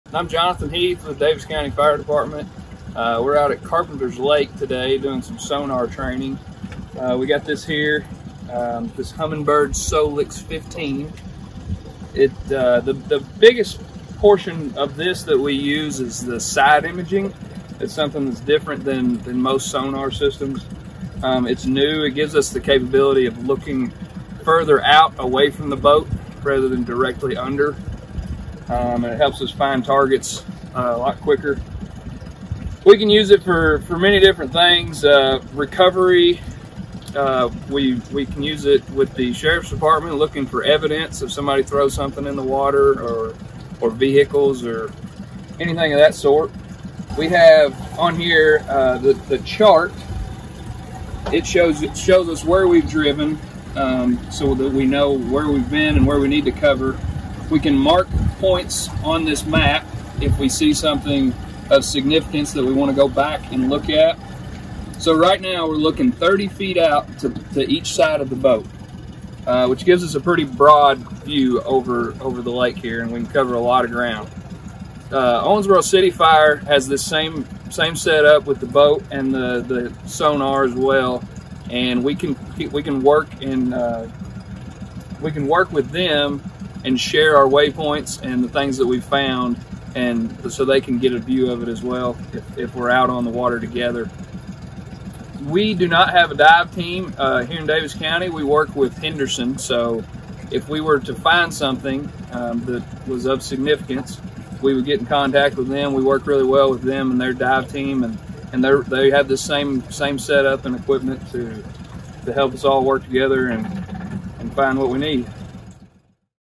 Carpenter Lake - Sonar Training
The Daviess County Fire Department participates in a training exercise at Carpenter Lake near Yelvington.

Firefighter Jonathan Heath highlights the capabilities of the Humminbird Side Imaging Sonar Program.  It is utilized by first responders for underwater search and recovery operations.